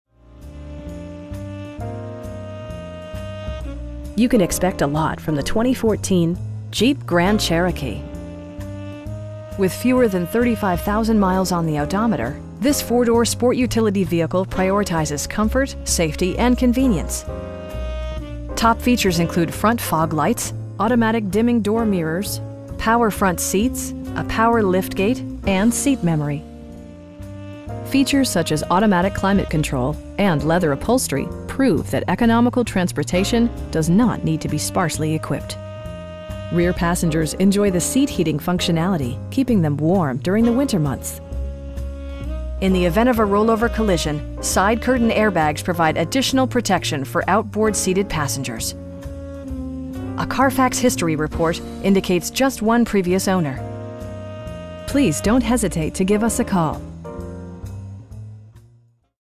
2014 Jeep Grand Cherokee Limited in Dulles, VA 20166
Don Beyer Volvo Of Dulles
21830 Pacific Blvd
in Dulles, VA 20166

You can expect a lot from the 2014 Jeep Grand Cherokee. With fewer than 35,000 miles on the odometer, this 4 door sport utility vehicle prioritizes comfort, safety and convenience. Top features include front fog lights, automatic dimming door mirrors, power front seats, tilt and telescoping steering wheel, a power liftgate, power windows, cruise control, and seat memory. Features such as automatic climate control and leather upholstery prove that economical transportation does not need to be sparsely equipped. Rear passengers enjoy the seat heating functionality, keeping them warm during the winter months. In the event of a rollover collision, side curtain airbags provide additional protection for outboard seated passengers. A Carfax history report indicates just one previous owner. We have the vehicle you've been searching for at a price you can afford. Please don't hesitate to give us a call.